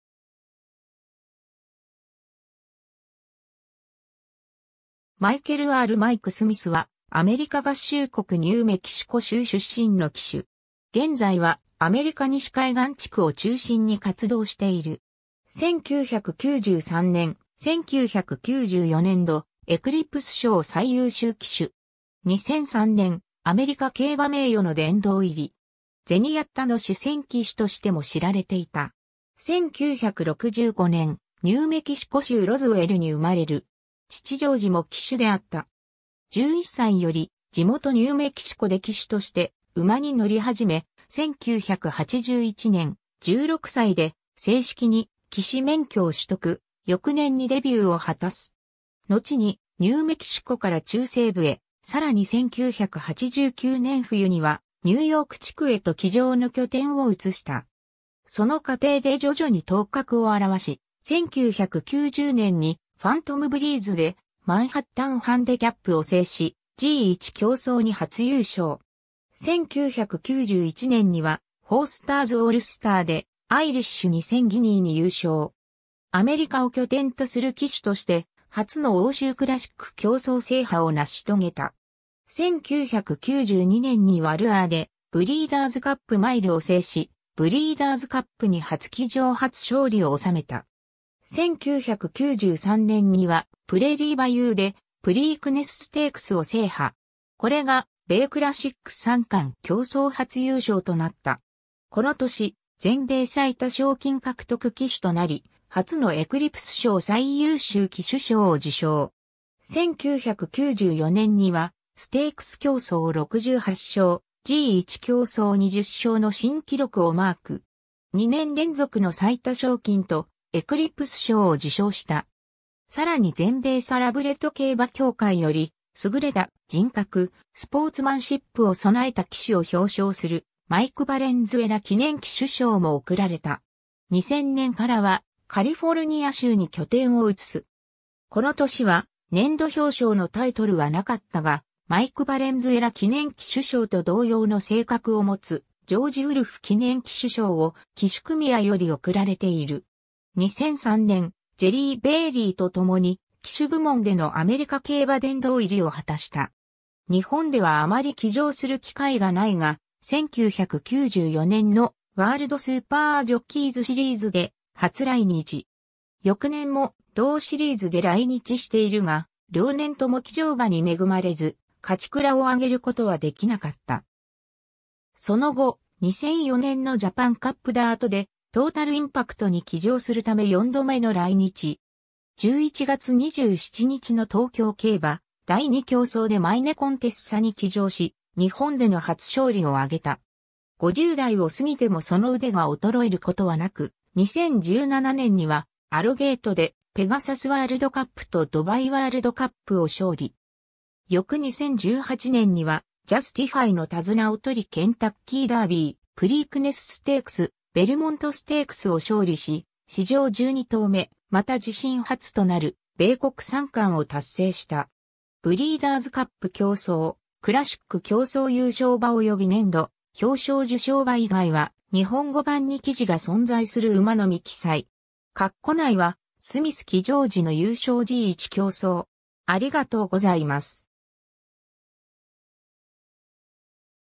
マイク・スミス
マイケル・アール・"マイク"・スミス（Michael Earl "Mike" Smith、1965年8月10日 - ）は、アメリカ合衆国ニューメキシコ州出身の騎手。
現在はアメリカ西海岸地区を中心に活動している。
1993年、1994年度エクリプス賞最優秀騎手。
2003年、アメリカ競馬名誉の殿堂入り。
ゼニヤッタの主戦騎手としても知られていた。
 1965年、ニューメキシコ州ロズウェルに生まれる。
父ジョージも騎手であった。
11歳より地元ニューメキシコで騎手として馬に乗り始め、1981年、16歳で正式に騎手免許を取得、翌年にデビューを果たす。
のちにニューメキシコから中西部へ、さらに1989年冬にはニューヨーク地区へと騎乗の拠点を移した。
その過程で徐々に頭角を現し、1990年にファントムブリーズでマンハッタンハンデキャップを制し、G1競走に初優勝。
1991年にはフォースターズオールスターでアイリッシュ2000ギニーに優勝。
アメリカを拠点とする騎手として初の欧州クラシック競走制覇を成し遂げた。
1992年にはルアーでブリーダーズカップ・マイルを制し、ブリーダーズカップに初騎乗初勝利を収めた。
 1993年にはプレイリーバユーでプリークネスステークスを制覇。
これが米クラシック三冠競走初優勝となった。
この年、全米最多賞金獲得騎手となり、初のエクリプス賞最優秀騎手賞を受賞。
1994年にはステークス競走68勝、G1競走20勝の新記録をマーク。
2年連続の最多賞金とエクリプス賞を受賞した。
さらに全米サラブレッド競馬協会より、優れた人格・スポーツマンシップを備えた騎手を表彰するマイク・ヴァレンズエラ記念騎手賞も贈られた。
 2000年からはカリフォルニア州に拠点を移す。
この年は年度表彰のタイトルはなかったが、マイク・ヴァレンズエラ記念騎手賞と同様の性格を持つジョージ・ウルフ記念騎手賞を、騎手組合より贈られている。
2003年、ジェリー・ベイリーと共に騎手部門でのアメリカ競馬殿堂入りを果たした。
 日本ではあまり騎乗する機会がないが、1994年のワールドスーパージョッキーズシリーズで初来日。
翌年も同シリーズで来日しているが、両年とも騎乗馬に恵まれず勝ち鞍を挙げることはできなかった。
その後、2004年のジャパンカップダートでTotal Impactに騎乗するため4度目の来日。
11月27日の東京競馬第2競走でマイネコンテッサに騎乗し、日本での初勝利を挙げた。
 50代を過ぎてもその腕が衰えることはなく、2017年にはアロゲートでペガサスワールドカップとドバイワールドカップを勝利。
翌2018年にはジャスティファイの手綱をとりケンタッキーダービー、プリークネスステークス、ベルモントステークスを勝利し、史上12頭目、また自身初となる米国三冠を達成した。
 ※ブリーダーズカップ競走、クラシック競走優勝馬および年度表彰受賞馬以外は、日本語版に記事が存在する馬のみ記載。
括弧内はスミス騎乗時の優勝G1競走。